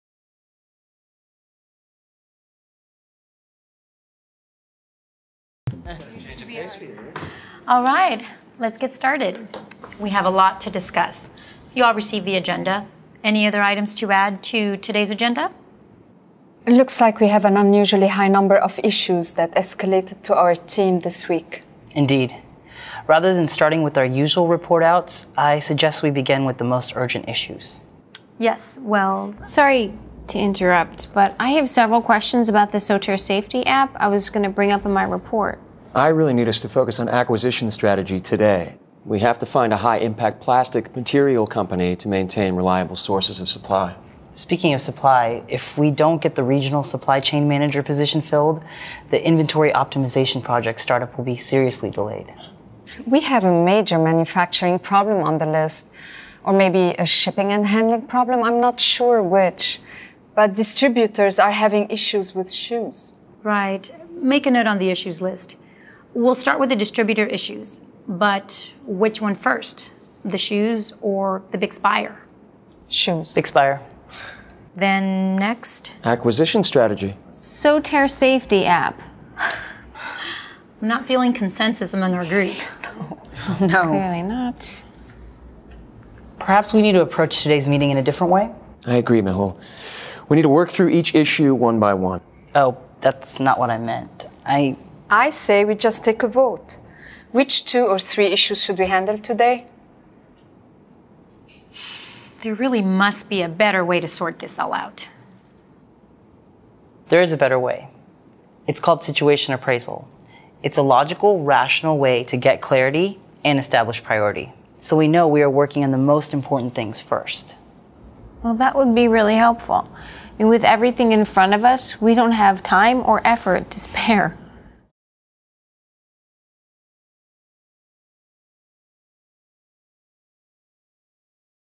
Sneak Preview: Video from Kepner-Tregoe to help prioritize critical issues
We're all involved in a seemingly never-ending cycle of meetings and conference calls.  Unfortunately, many of these seem to have no focus or agenda. We discuss the challenges facing the business, but everything seems to be a priority and nothing ever gets really resolved. These kinds of meetings can go on forever with nothing really getting accomplished and they turn into a waste of time and effort.

But what if there was a process that would ensure that our conversations focused on only the most impactful issues at hand and helped our teams communicate more effectively and be more productive?

The good news is that there is such a tool. It's called a Situation Appraisal and is part of Kepner-Tregoe's Clear Thinking processes. A Situation Appraisal is where you take inventory of all the things that require you and your team's attention: a supply chain or manufacturer issue, a competitive threat, a potential merger or acquisition, etc. By following the Situation Appraisal process, you'll see issues prioritized more effectively than ever and specific next steps defined so everyone has a clear understanding.

To show practical applications of Situation Appraisal and other components of KT Clear Thinking, as part of the recent update to the KT Problem Solving & Decision Making program we introduced a series of new videos and case studies. We leveraged the most current media formats including live-action video, illustrated motion-graphics and animation to enhance the learning experience. You can see a sneak preview of one of those videos below.  The scene features a Daily Meeting Dilemma: a global leadership team struggling with a variety of critical issues and no clear path forward. That is until they apply KT Situation Appraisal.